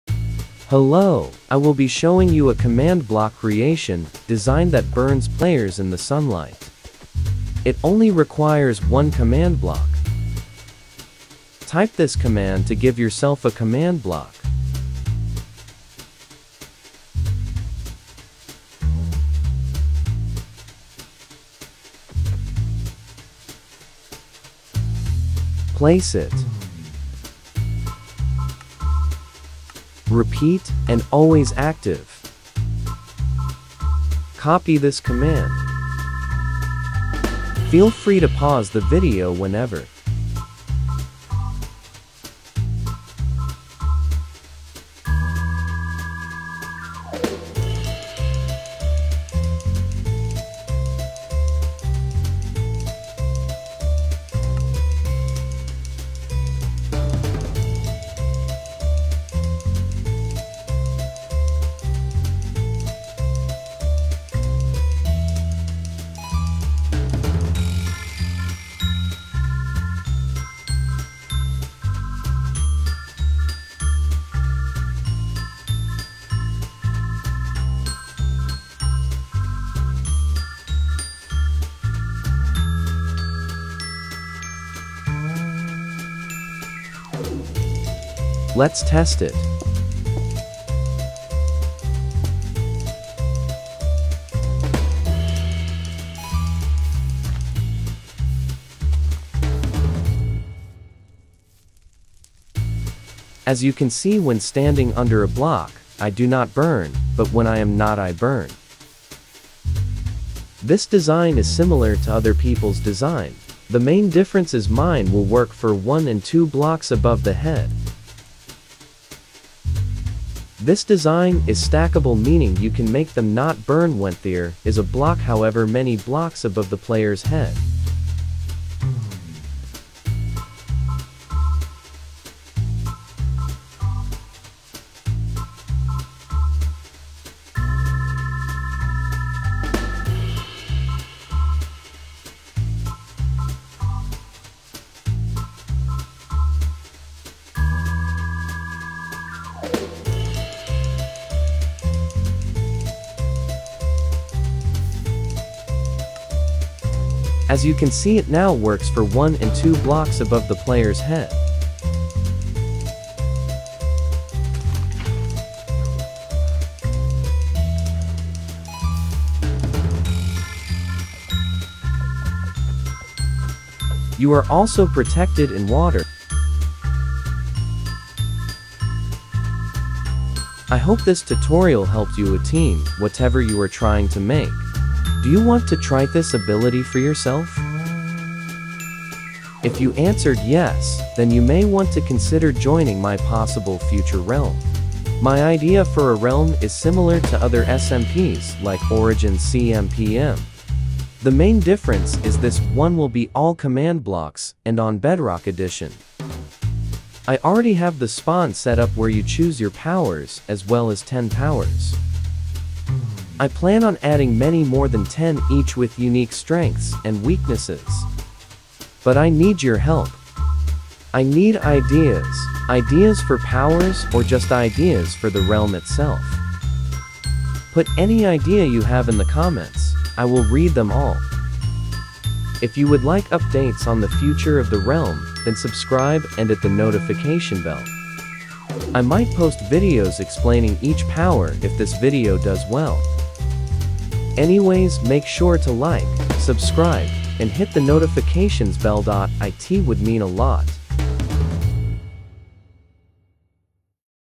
How to make players burn in the daylight in one command block! (PE, Xbox, PlayStation, Windows 10)
The command: execute @a ~~~ detect ~~2~ air 0 execute @a detect ~~3~ air 0 fill ~~~ ~~~ fire 0 replace air

Alternatively you could use two command blocks but instead of “fill” one has particle and one either withers or does the damage command.